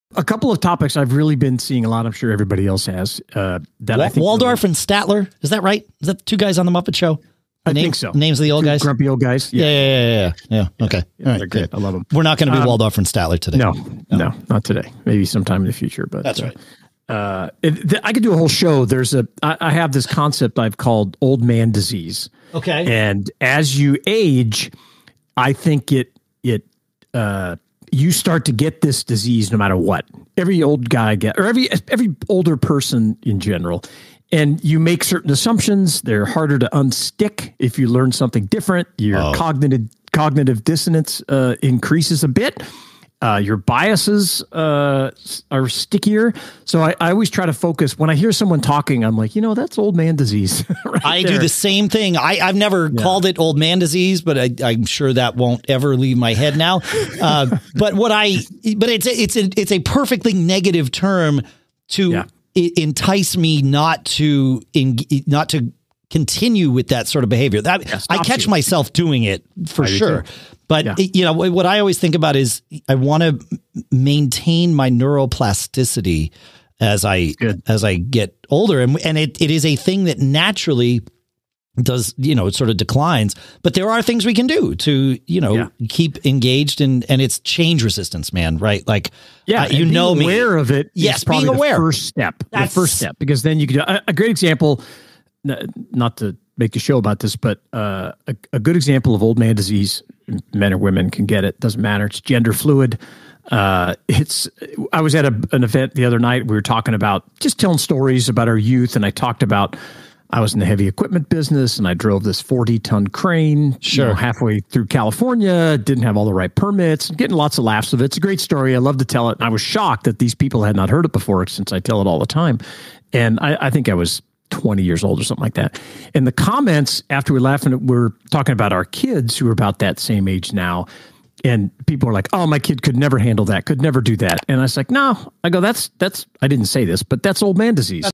Old Man Disease
It affects us all as we get older.
Check out this mini-segment from Episode 351 where Shannon and Dave discuss why learning everyday and the neuroplasticity of your brain are important fighting what we call the old man disease.